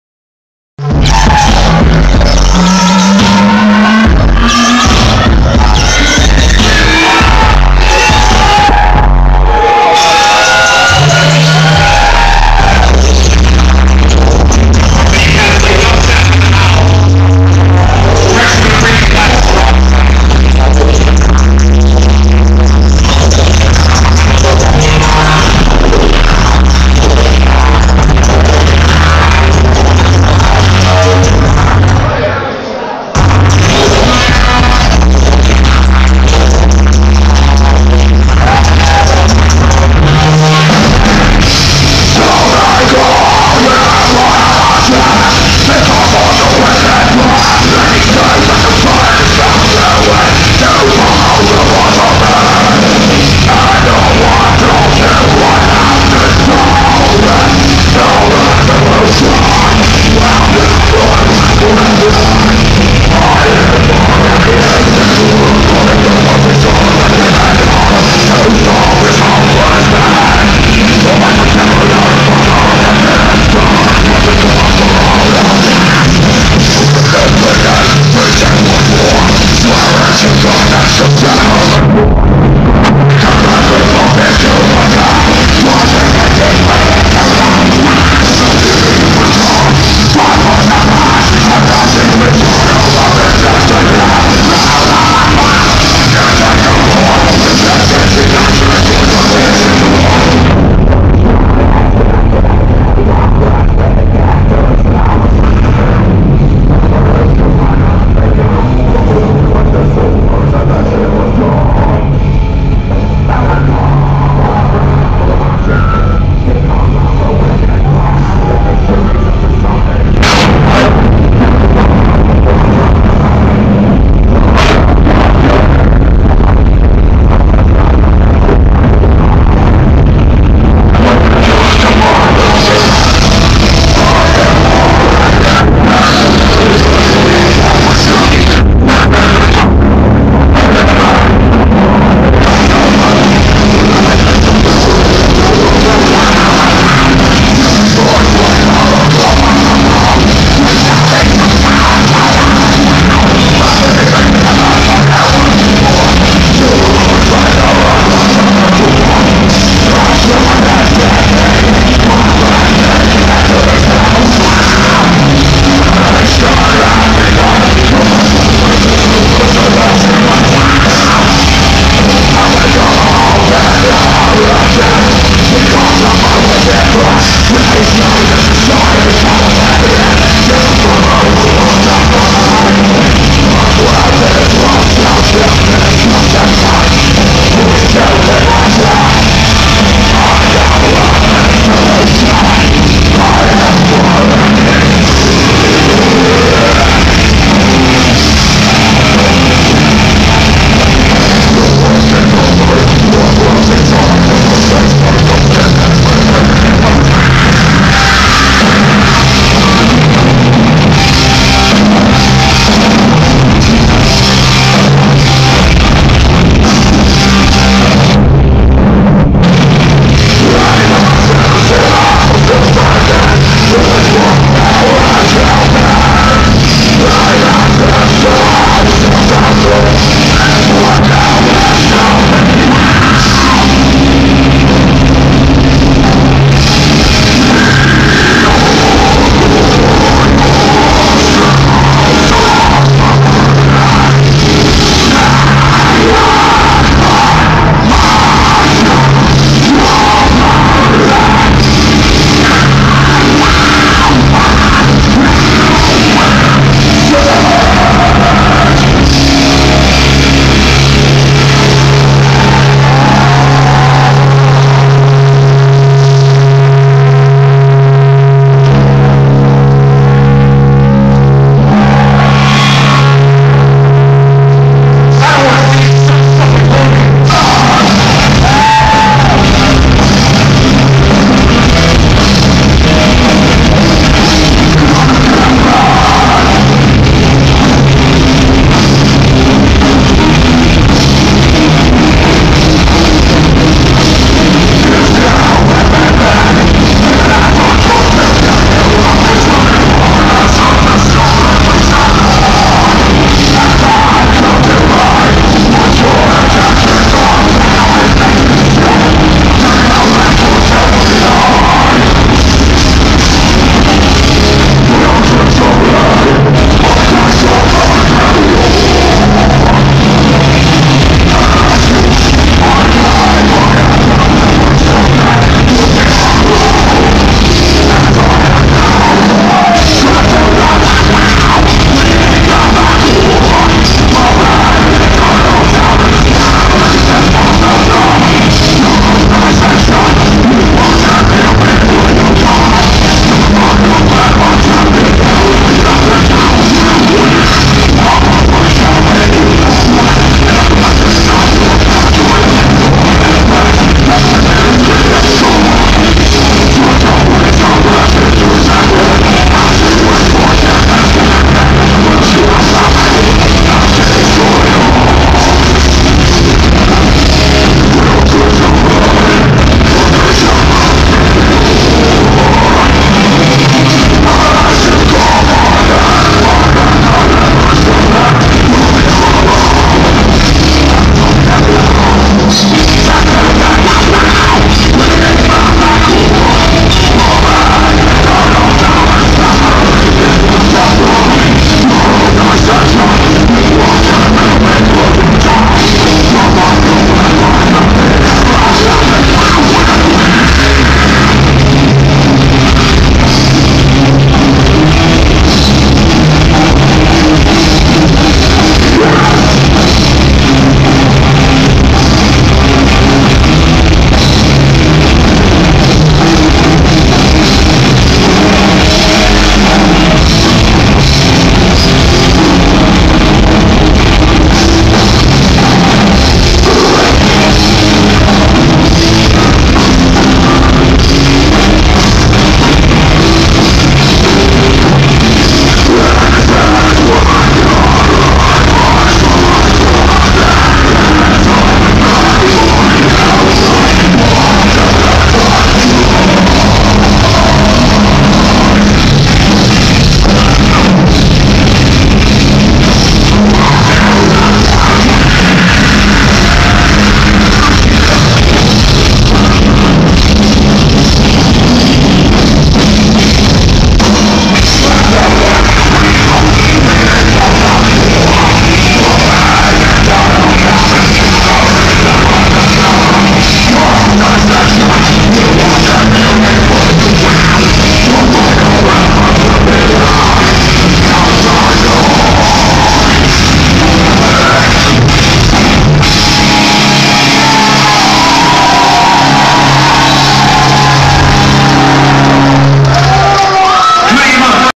We Kill Kids Live! 4/13/12
We Kill kids at the Orange Peel in Asheville, NC. East Coast Metal OCT!!! Sorry for putting my thumb on mic near beggining. Look for the mosh pit action.